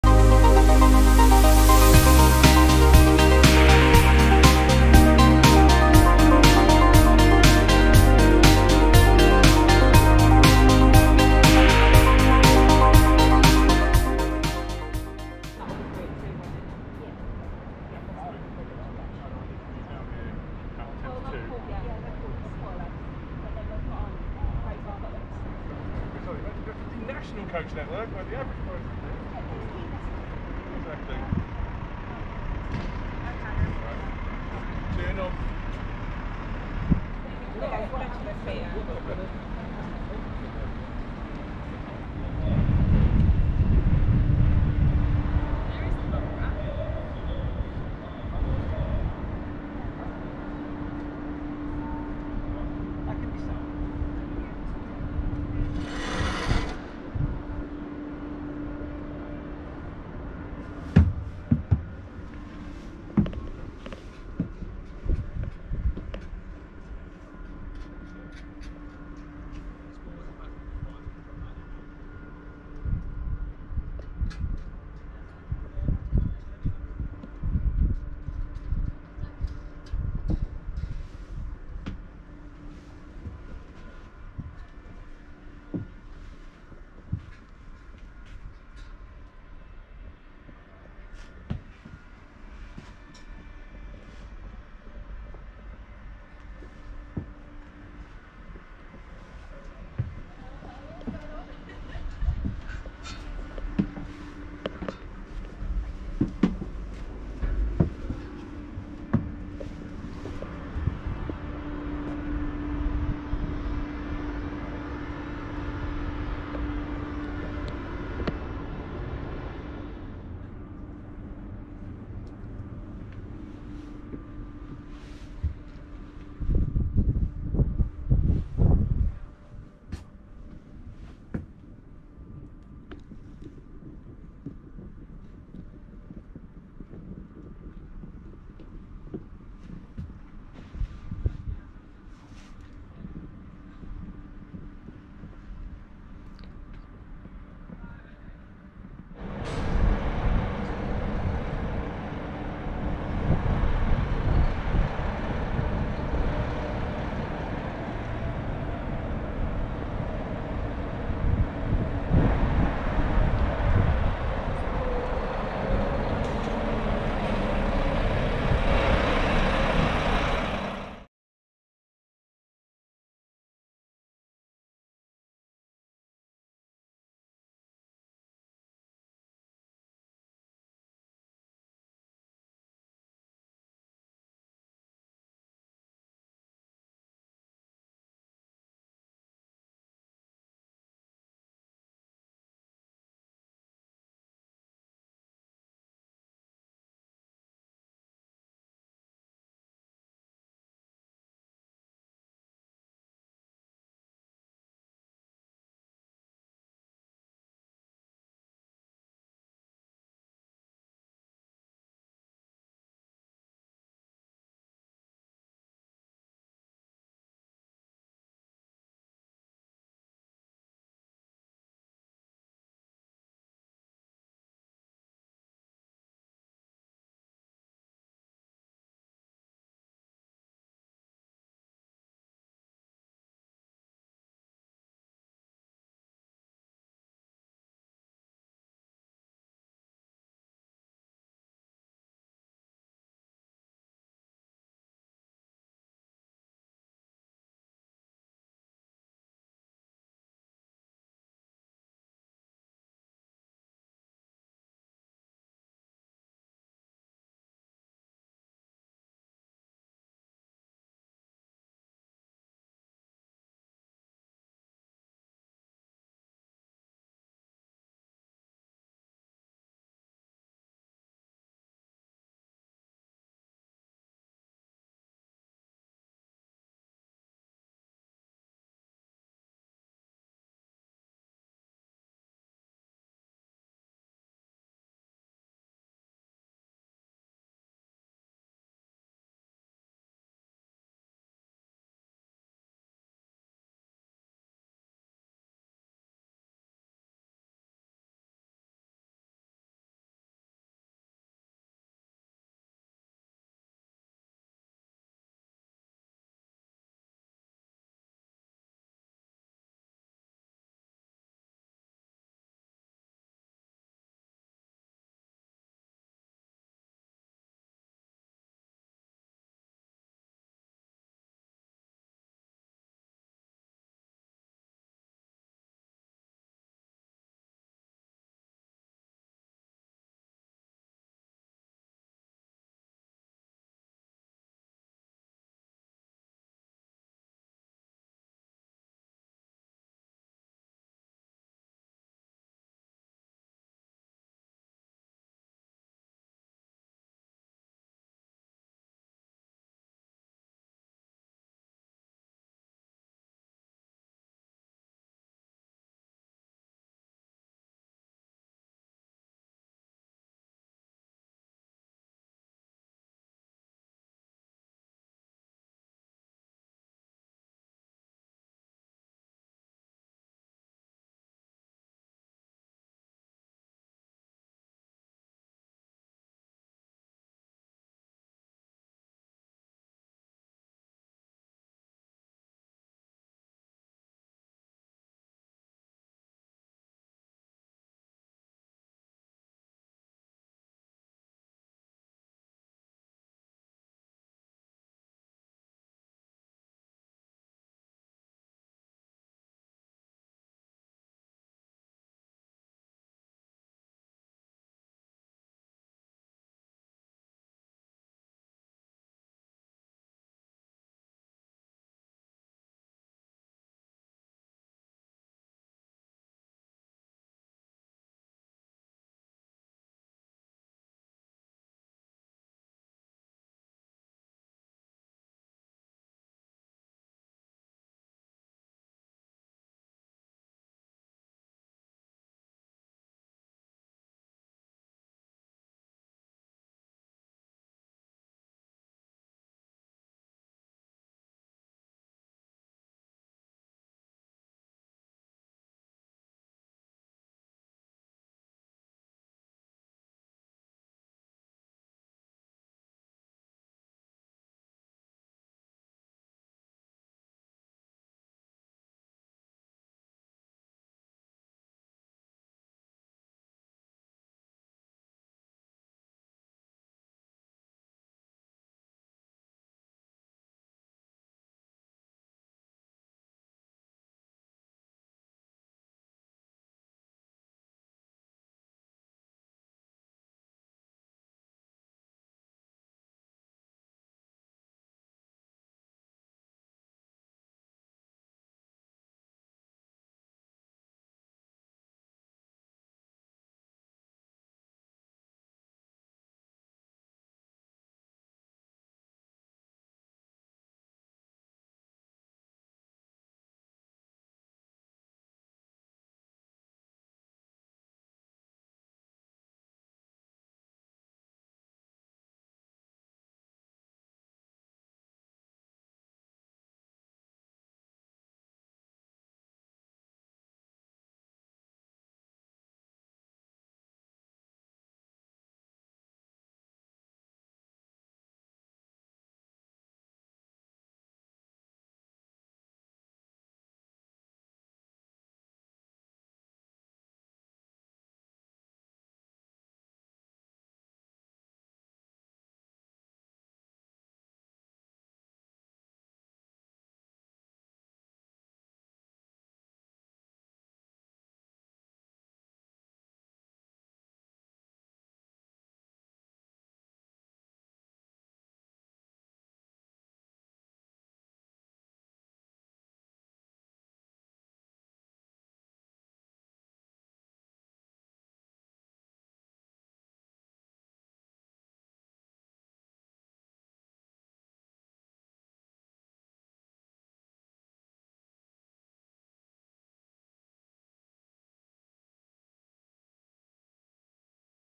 Event | National Express 50th Anniversary
In June, National Express held an event at Wembley Stadium to mark its 50th anniversary. It launched a new book charting its half century, from National Bus Company ownership to PLC status, and displayed three generations of iconic white coaches. Alongside the latest Caetano Levante 3 was a Royal Blue ECW-bodied Bristol RELH6G which could be seen on the network in the 1970s and a Leyland Leopard with Plaxton Supreme bodywork, new in 1980.